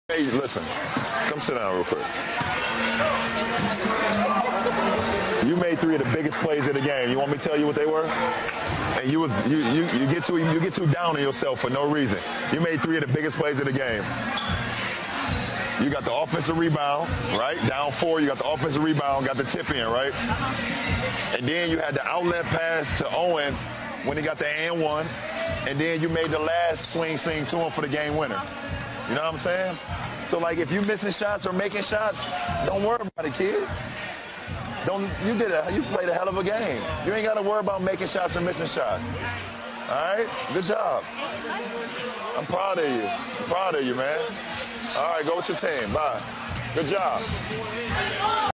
Video viral: la emotiva lección que le dio LeBron James a su hijo de 11 años
LeBron James, actual estrella de Los Ángeles Lakers y considerado como uno de los mejores basquetbolistas de la historia de la NBA, subió a su cuenta de la red social Instagram un video que rápidamente se volvió viral.
The King, al notar la tristeza y fastidio que sentía su hijo Bryce Maximus -de 11 años- por fallar una canasta clave en el encuentro, se acercó y le dio una emotiva lección.
"Has hecho tres de las mejores jugadas del partido. ¿Quieres que te diga cuáles fueron?. Te desanimaste demasiado sin ninguna razón. Hiciste tres de las mejores jugadas del partido. Cogiste el rebote de ataque cuando perdías de cuatro y conseguiste el palmeo, ¿cierto?. Luego le diste el pase a Owen cuando consiguió el 2+1 e hiciste el cambio de posición para el tiro ganador. ¿Sabes lo que estoy diciendo? No te preocupes por si fallas o metes los tiros, chico. Has jugado un partidazo, no te preocupes por ello. Buen trabajo, estoy orgulloso de ti. Ve con tu equipo, buen trabajo", fueron las palabras que esbozó el ganador de tres anillos de la NBA (dos con Miami Heat y uno con Cleveland Cavaliers).
Bryce Maximus James no es el único hijo del alero que busca triunfar en el básquet. LeBron Jr -el mayor de la dinastía, con 14 años- es considerado por los medios especializados como uno de los mejores de su generación.